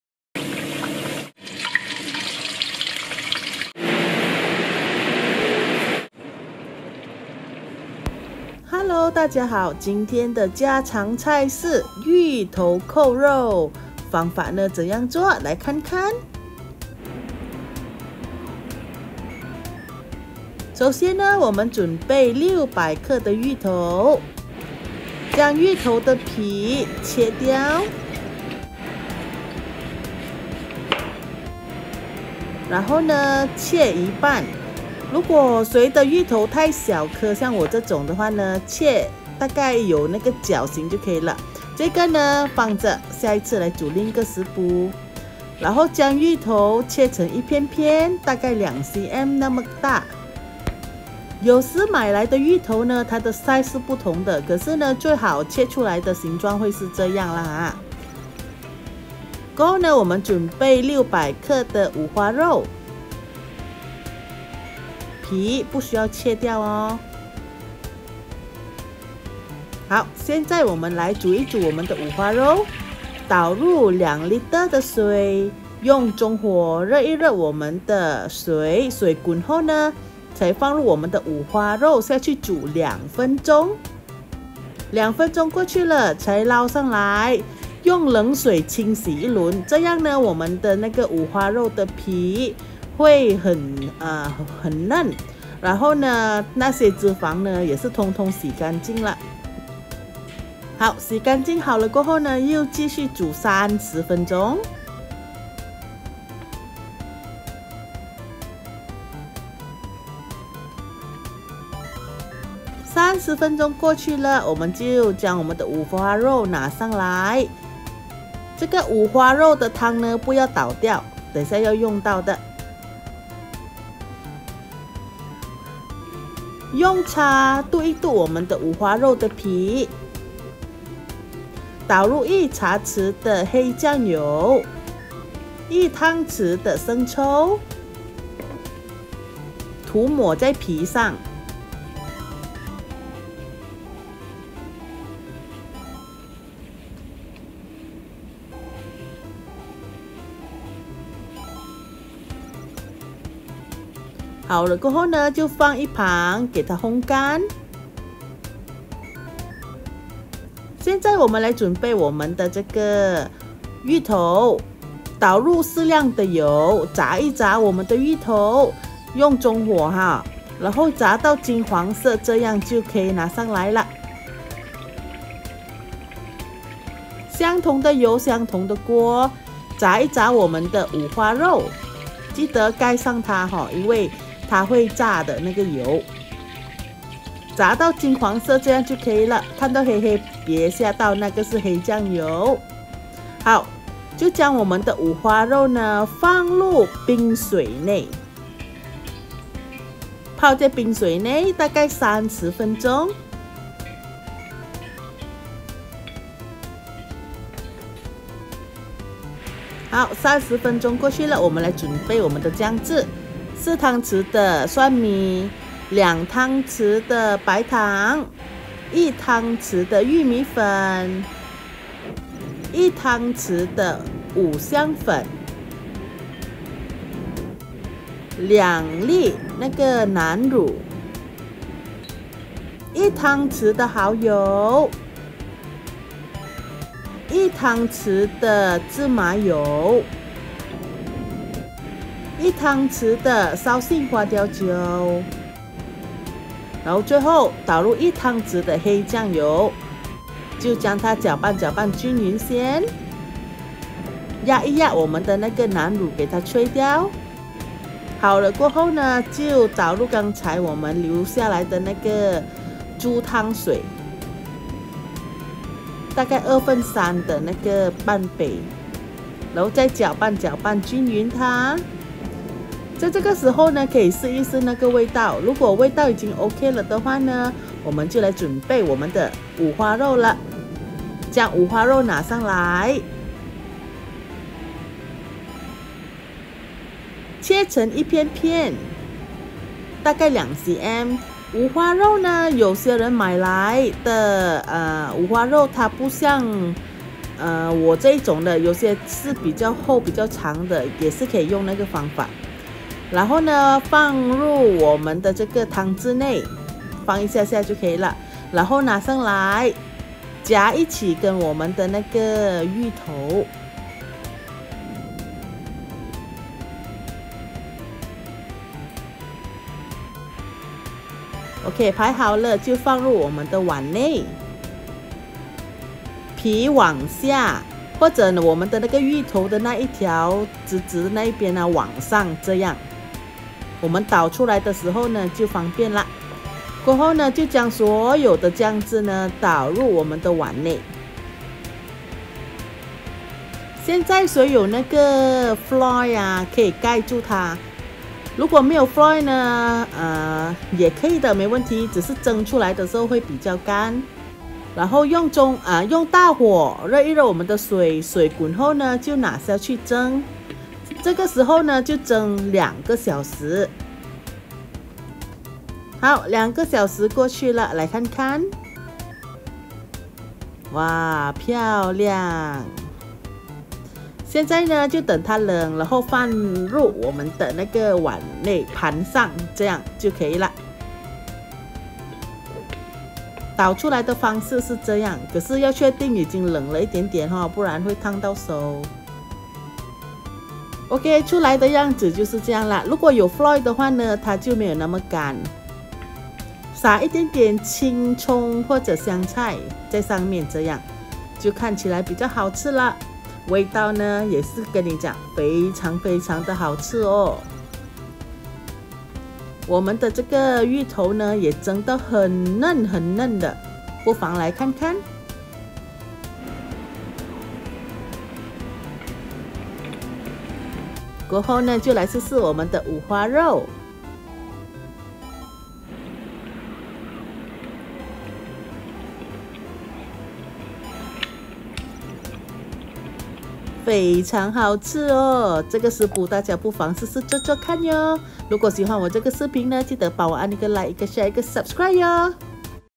芋头扣肉｜How to cook Steamed Pork Belly with Taro
原料：
 600克芋头
 600克五花肉

 酱汁：
 4汤匙大蒜
 1&1/2汤匙糖
 1汤匙玉米淀粉
 1汤匙五香粉
 2&1/2立方红腐乳
 1汤匙蚝油
 1汤匙芝麻油
 1汤匙绍兴酒
 1汤匙黑酱油
 2/3杯肉汤

Steamed Pork Belly with Taro
In this video, I'll share my mom's Steamed Pork Belly with Taro/Kao Bak Recipe with you. Hope you like it.😊

Ingredients:
600g Taro
600g Pork Belly

The sauce:
4Tbsp garlic
1&1/2Tbsp Sugar
1Tbsp Corn Starch
1Tbsp Five-spice Powder
2&1/2cubes red fermented bean curd
1Tbsp oyster sauce
1Tbsp sesame oil
1Tbsp Shaoxing Wine
1Tbsp dark soy sauce
2/3 cup meat stock